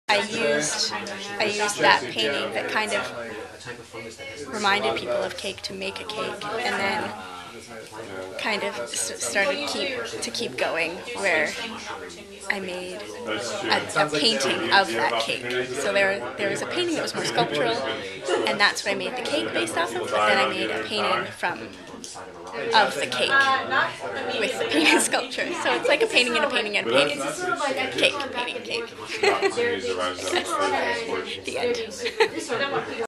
Leah Rosenberg: Painting in a Cake
Leah Rosenberg talks about her art cakes at Dinner Discussion #15, a gathering organized by Leif Hedendal.